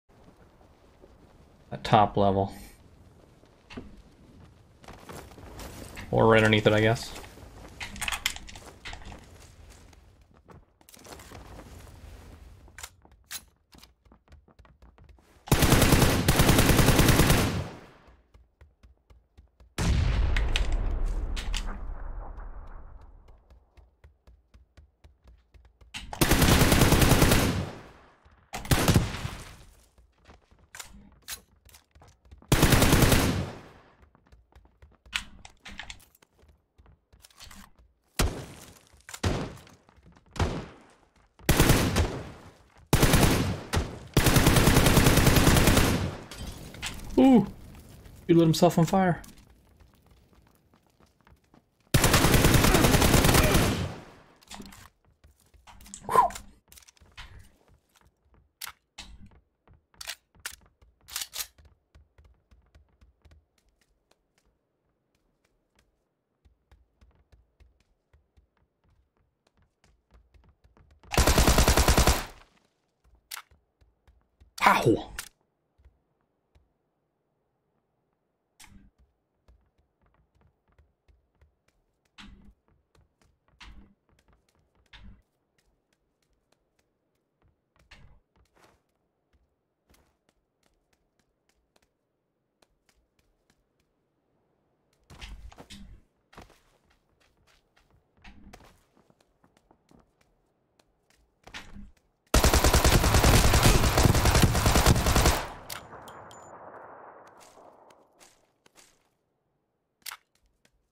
6 Kill Hacienda Drop!
This was a crazy little start to my first game of the day.